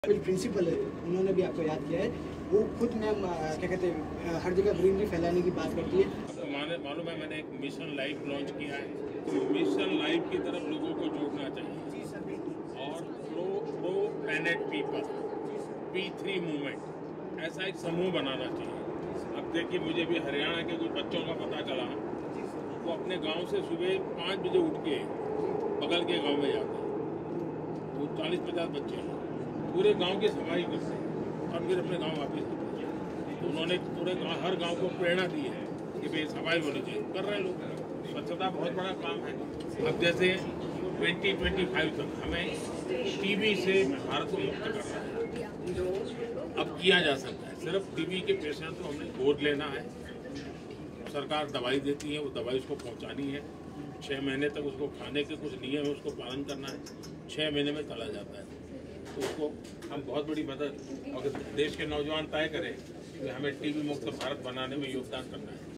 Swachhata, Mission LiFE & TB Mukt Bharat... What PM Modi discussed with students aboard Delhi metro
In a memorable moment, PM Modi interacted with students and fellow commuters aboard the Delhi Metro. PM Modi was travelling to the DU to take part in the centenary celebrations of the University.

Prime Minister Narendra Modi travels to Delhi University by metro